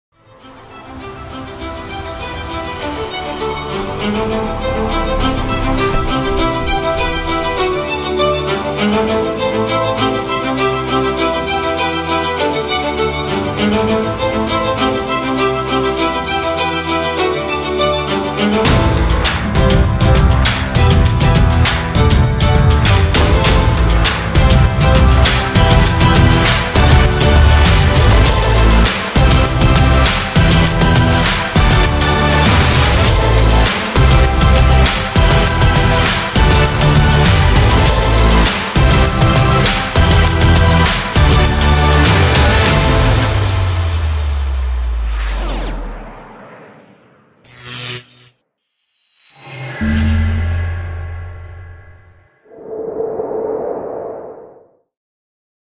Solotech Behind The Scenes - Soft Goods Workshop - Toronto
The rock wall recreates with fabric the look of a massive and majestic geological structure. As the panels themselves are modular our in-house team can create a custom sized backing upon which to affix them. Give your event a sense of natural wonder with this awesome display.